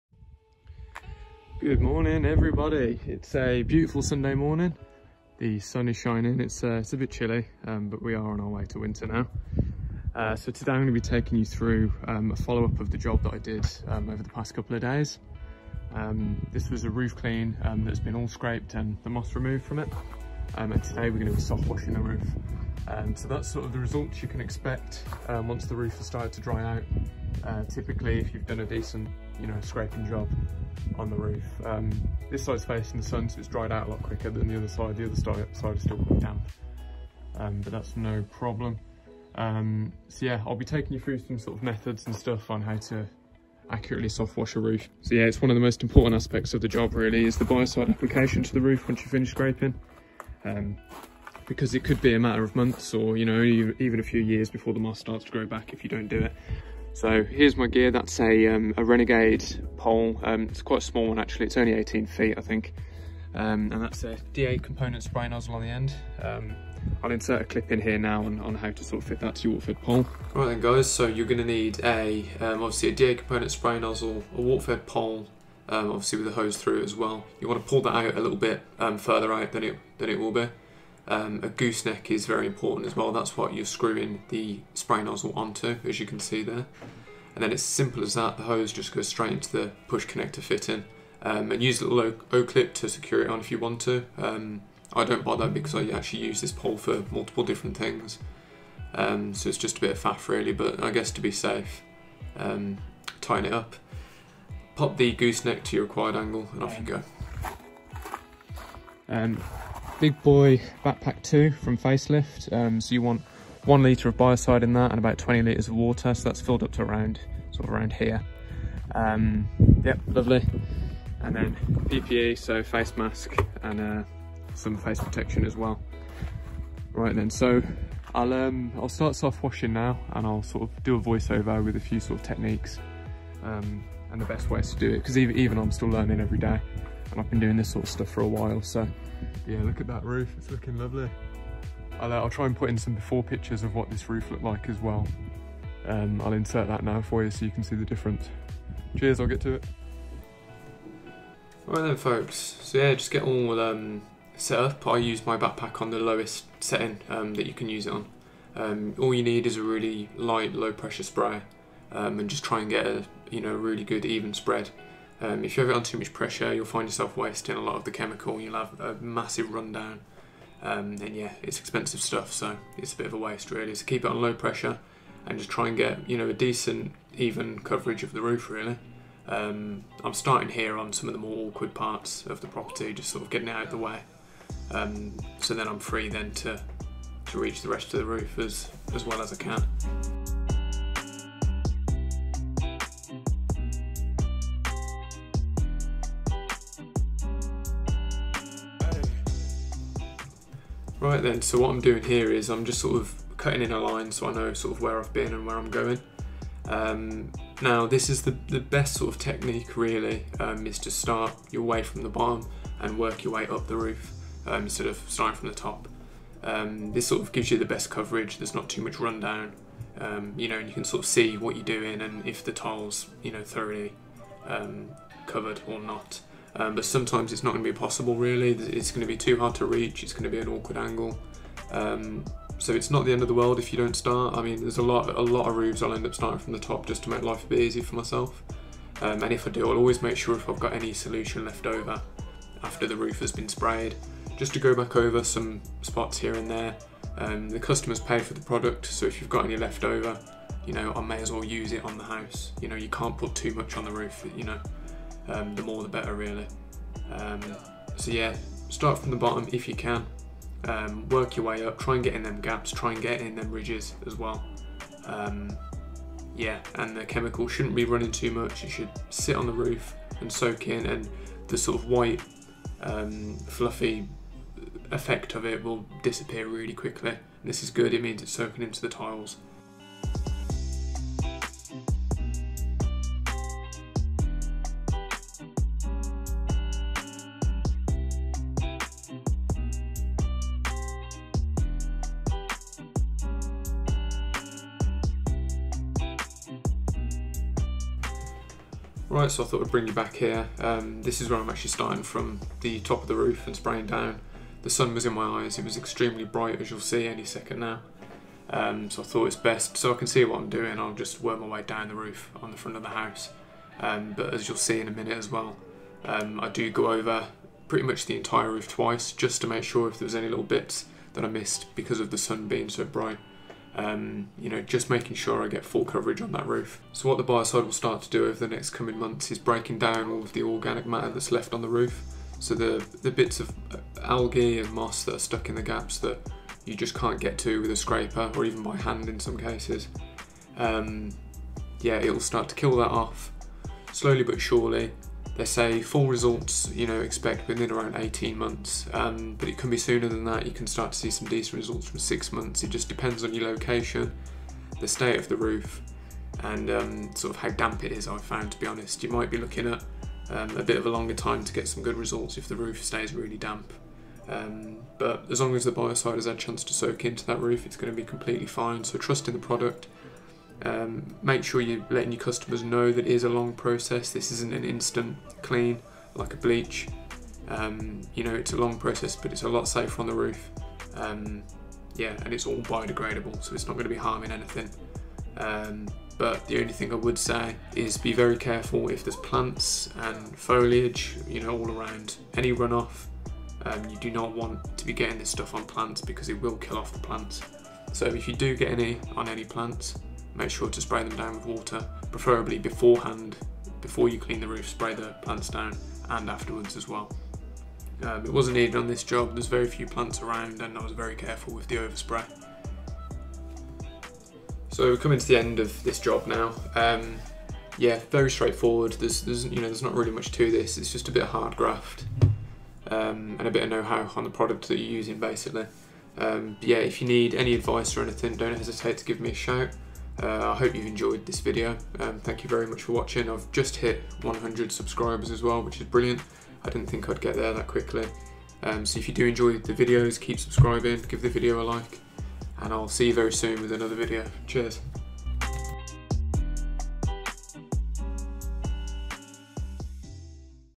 How To Use Biocide To Softwash A Roof // Partridge Exterior Cleaning
Complete and fairly thorough run down on everything you need to softwash a roof using biocide.
Using Algo Clear Pro,
Facelift Bigboy Backpack 2
Renegade WFP
DA components Spray nozzle.

Facebook.com/partridgeexteriorcleaning